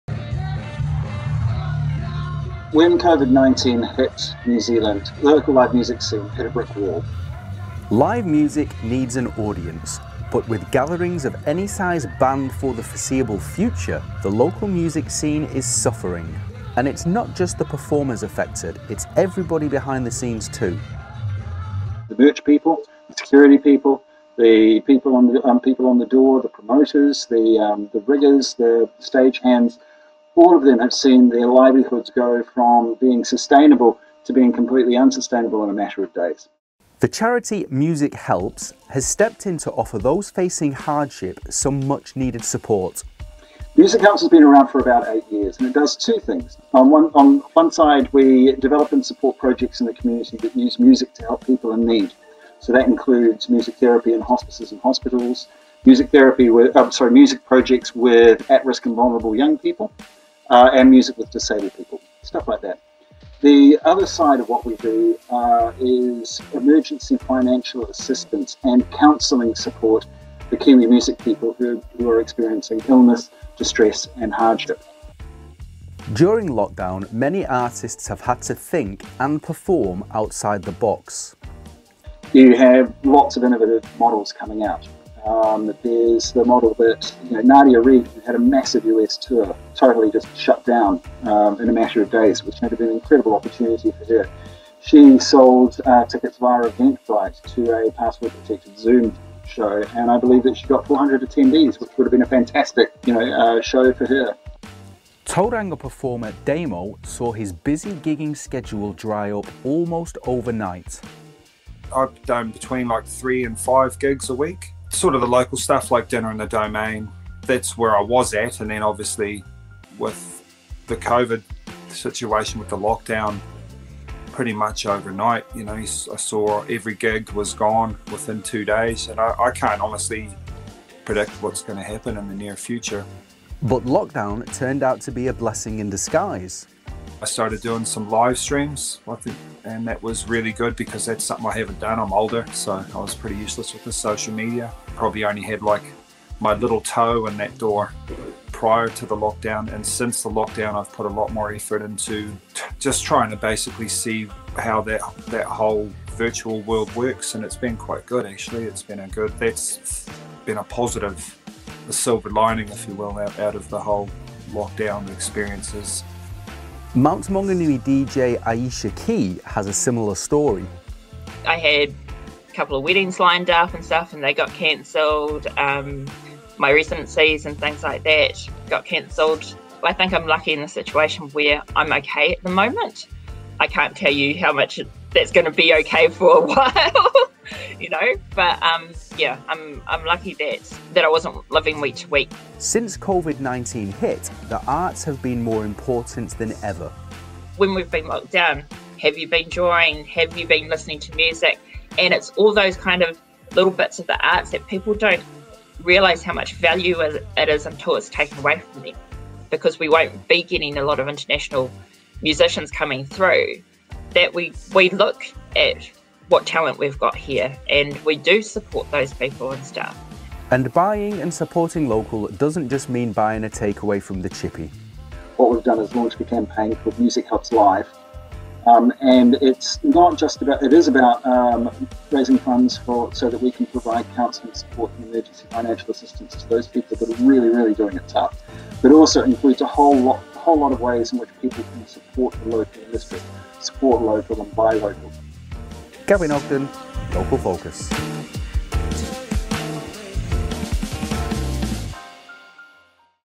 Music scene hits 'a brick wall' | Local Focus
Gigging musicians adapt in quarantine.